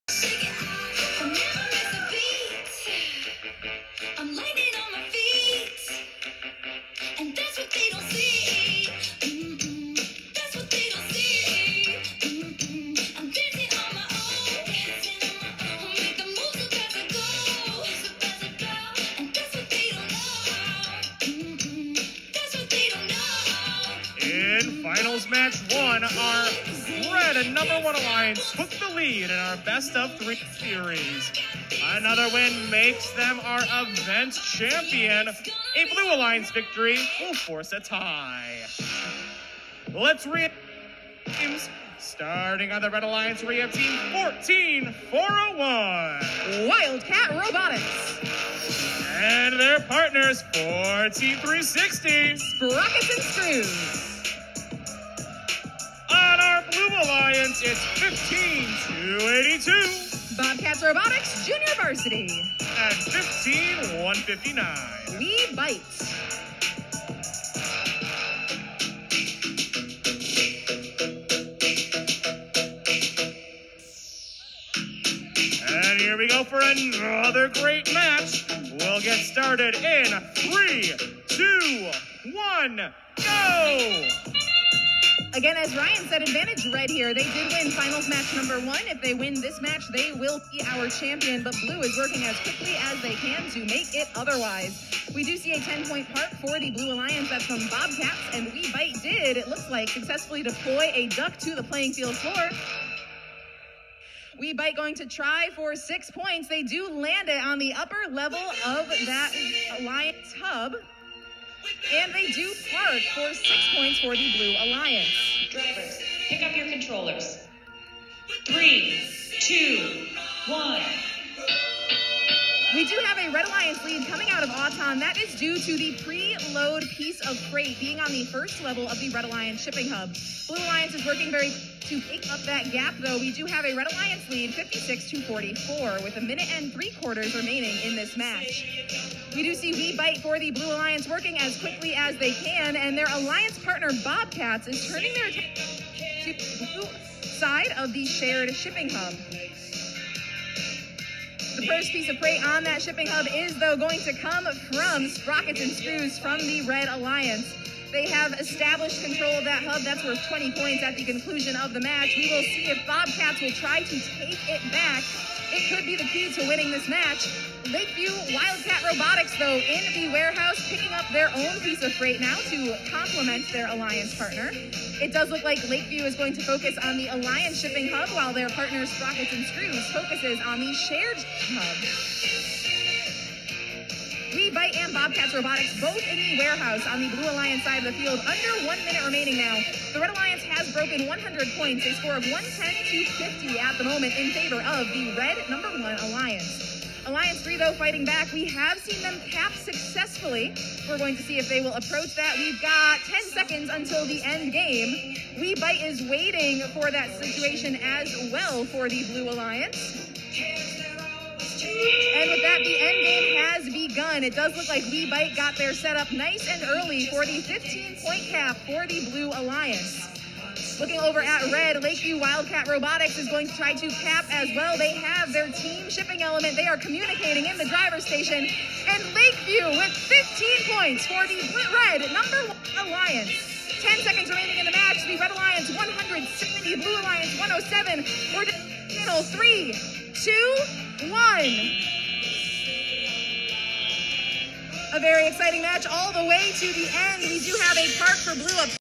Horizon Finals Match 2 2023
This was the match that propelled our team to the State Championship for the first time.  Thanks to Sprockets and Screws for being a fantastic alliance partner.